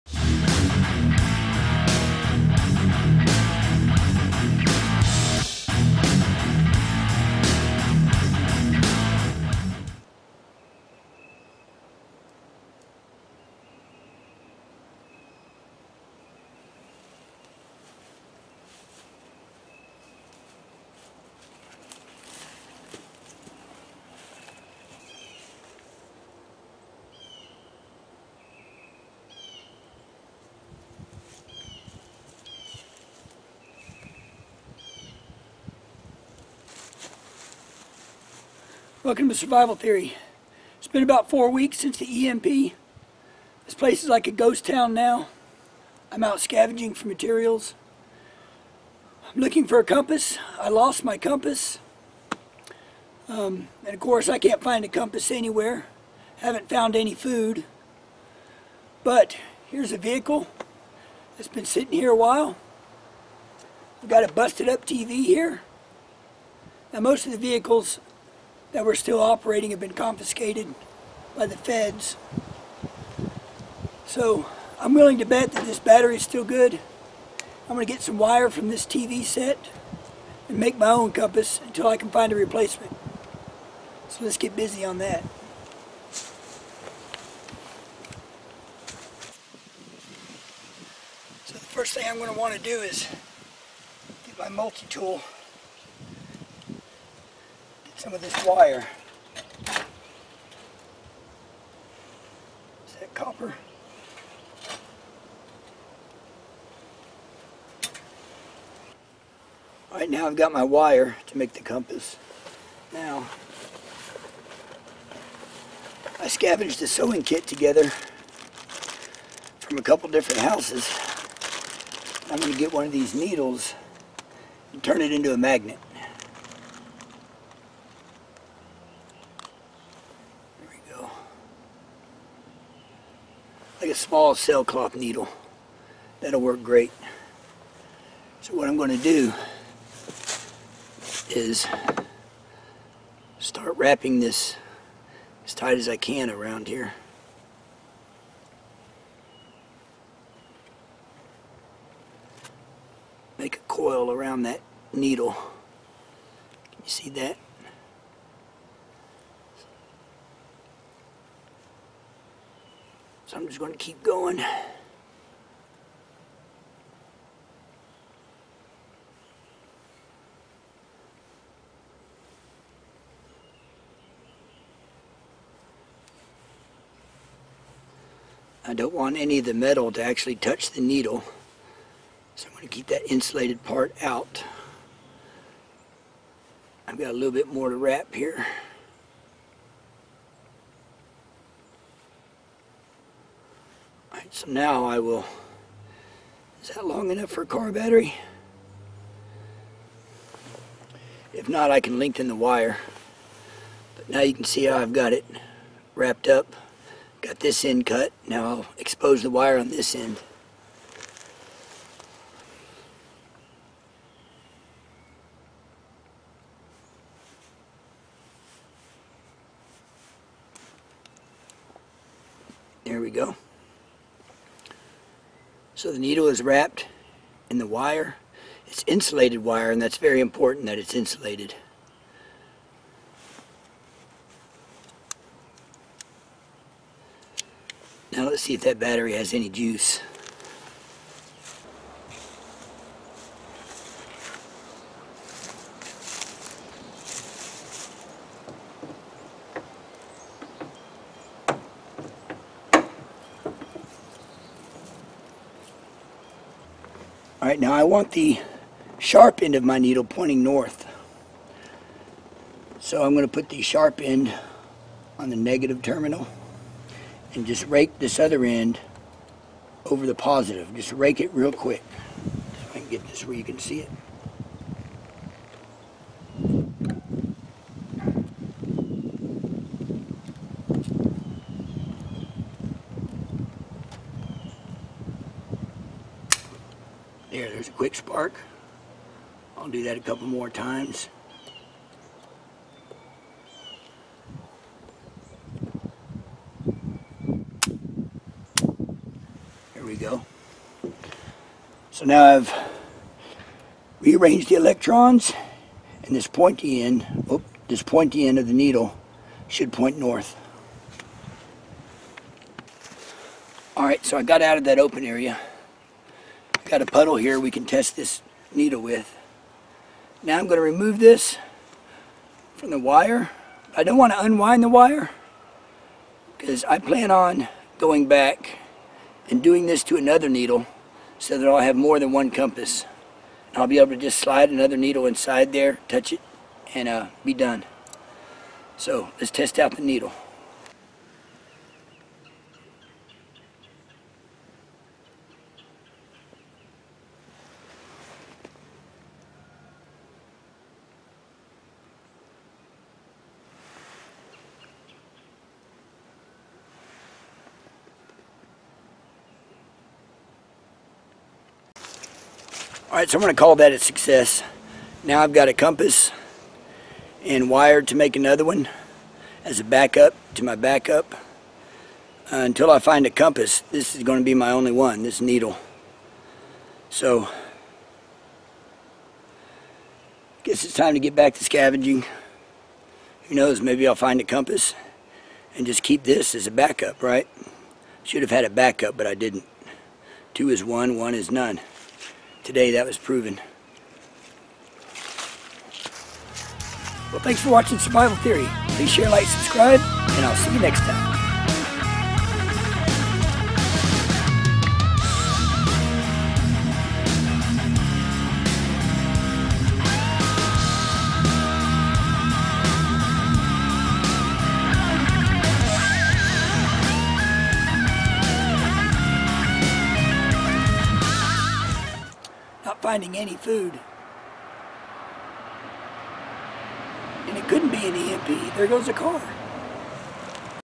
Electric Survival Compass
Part of the map and compass series, this episode looks at how to make a survival compass.
Best prepping, survival, and camping food on the market. Its called NUTRIENT Survival for a reason.